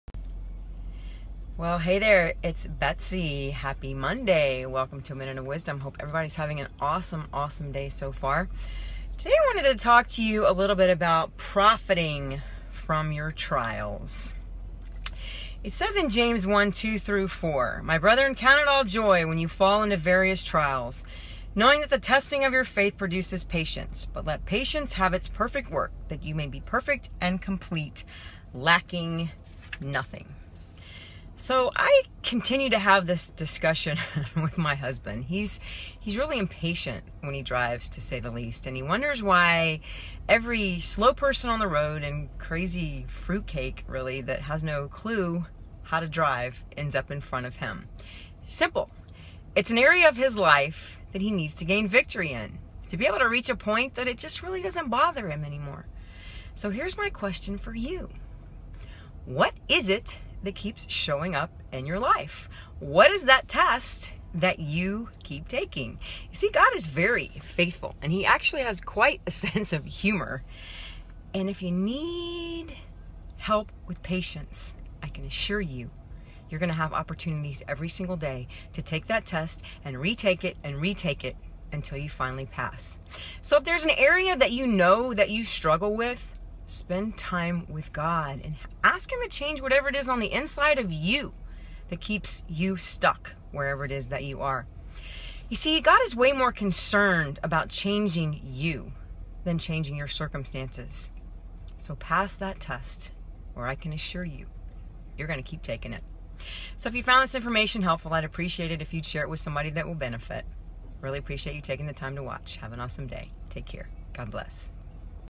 How to Profit From Trials...
What is it that keeps showing up in your life...what's the test you keep taking?  You will have opportunities every day to take that test until you finally pass.  God is way more concerned about changing you than changing your circumstances...so pass that test...or can assure you that you will keep taking it.